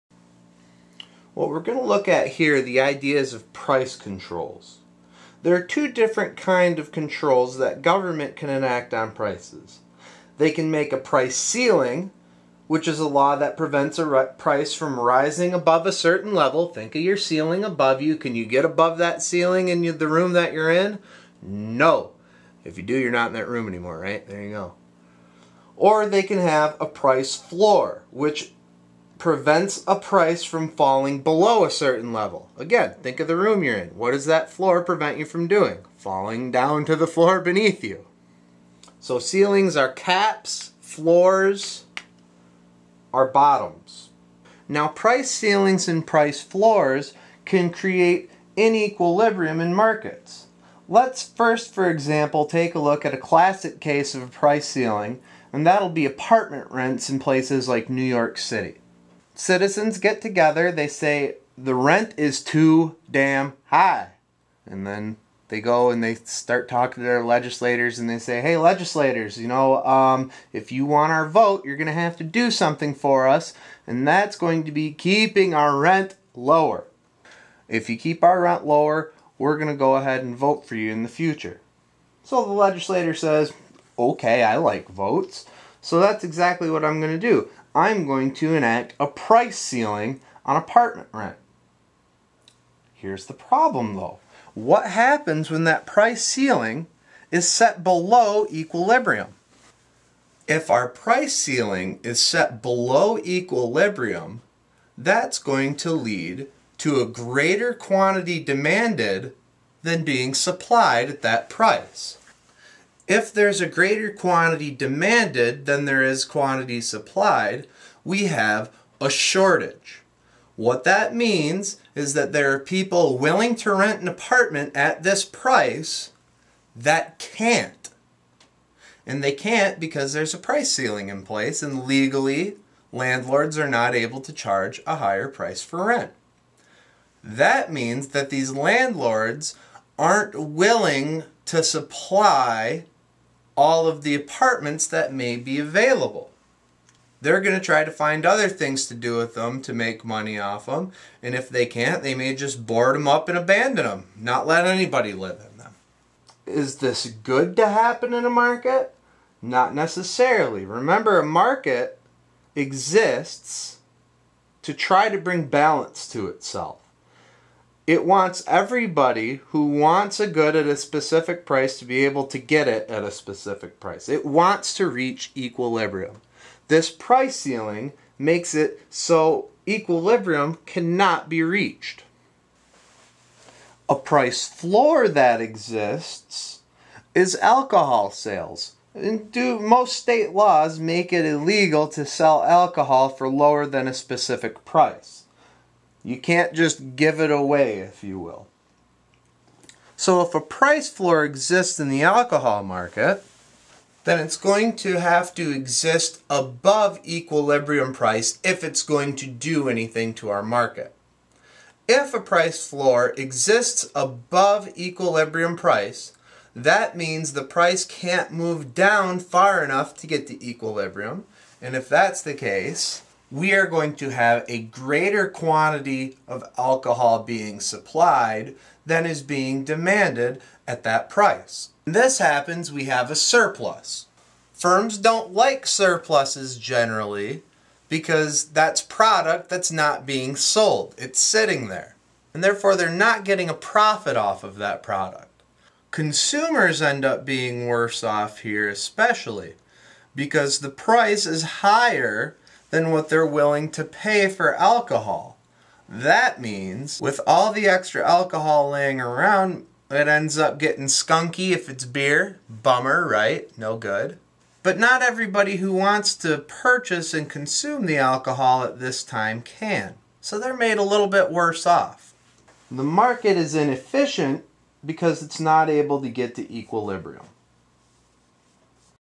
Price Controls: Breaking the Glass Ceiling
The efforts of the market to reach equilibrium can be thwarted by price controls.